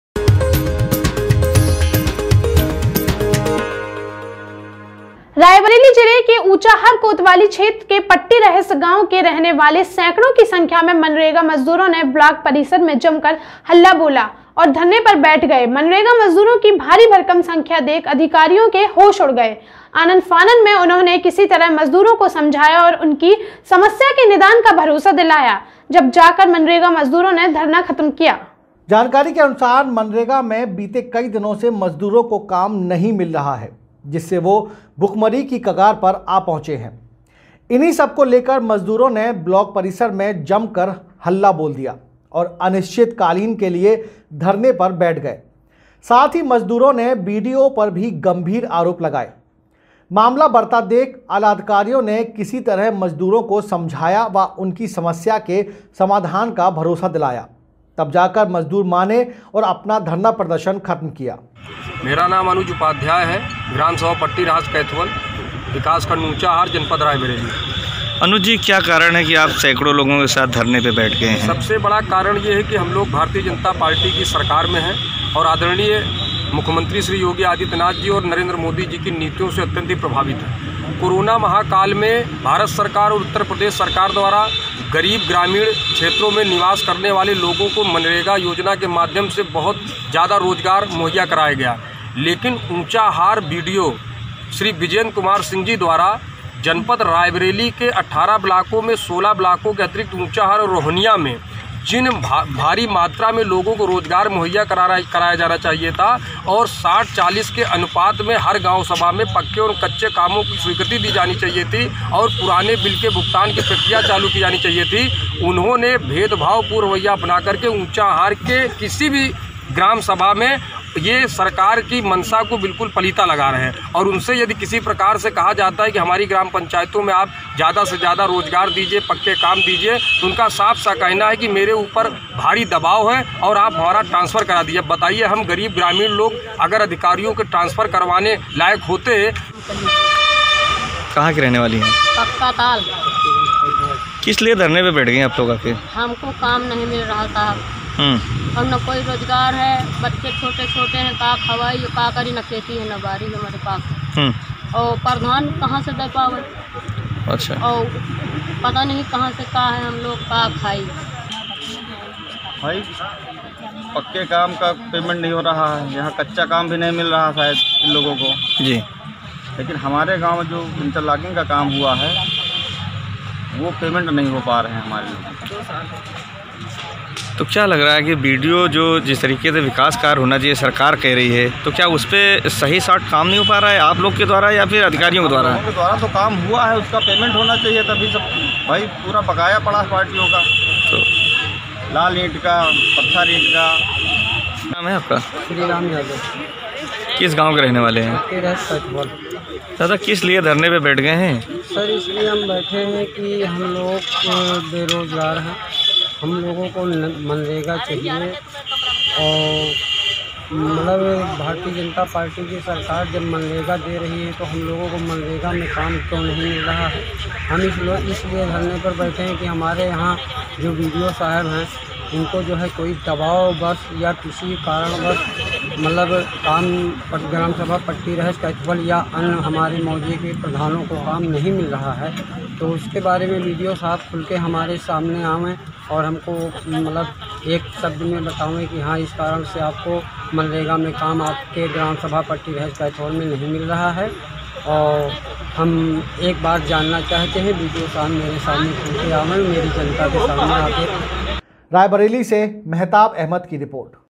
काम न मिलने से परेशान मनरेगा मजदूरों ने ब्लॉक परिसर में किया धरना प्रदर्शन।
रिपोर्ट-  महताब अहमद